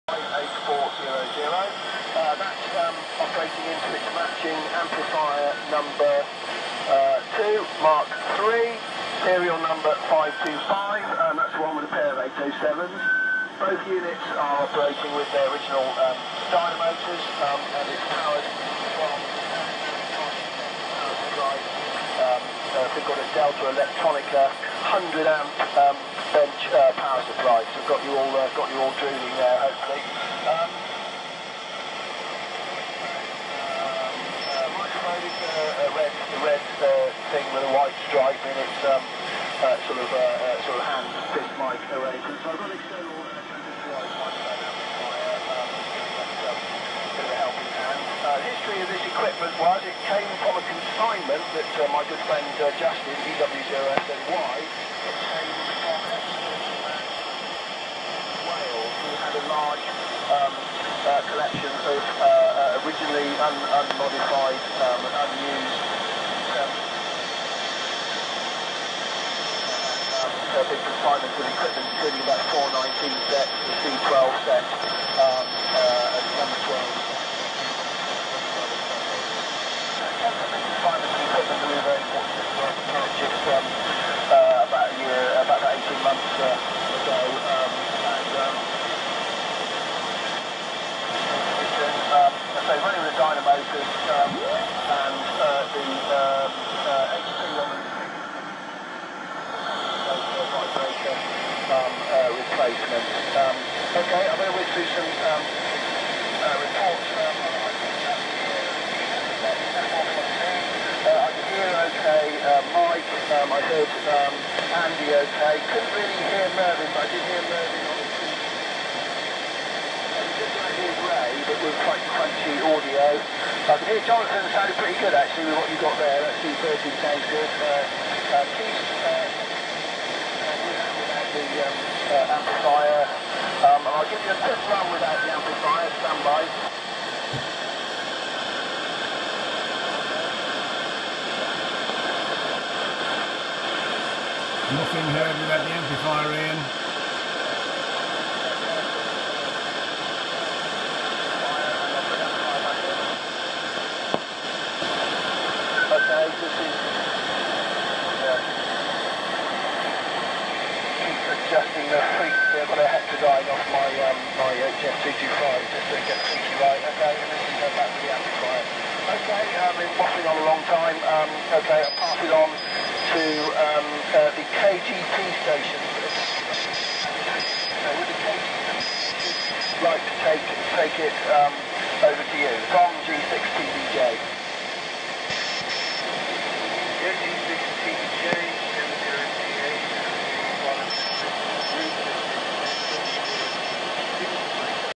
Eddystone 730/4 Vintage Military Receiver receiving the VMARS Saturday morning net
The second of 2 videos of the Eddystone 730/4 Vintage Military Receiver in action receiving the Saturday Morning net of the Vintage Military Amateur Radio Society.

As of the 16th July 2020 this receiver is for sale on eBay